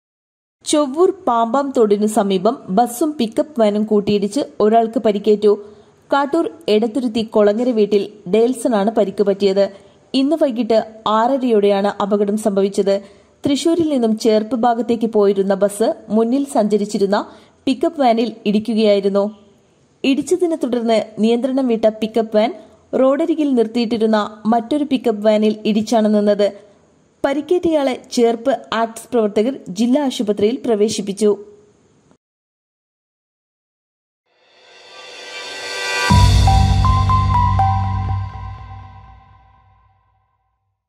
ബസ്സും പിക്കപ്പ് വാനും കൂട്ടിയിടിച്ച് ഒരാൾക്ക് പരിക്കേറ്റു
ചെവ്വൂർ പാമ്പാം തോടിനു സമീപം ബസ്സും പിക്കപ്പ് വാനും കൂട്ടിയിടിച്ച് ഒരാൾക്ക് പരിക്കേറ്റു. കാട്ടൂർ എടത്തിരുത്തി കൊളങ്ങര വീട്ടിൽ ഡേൽസൺ(21) ആണ് പരിക്ക് പറ്റിയത്.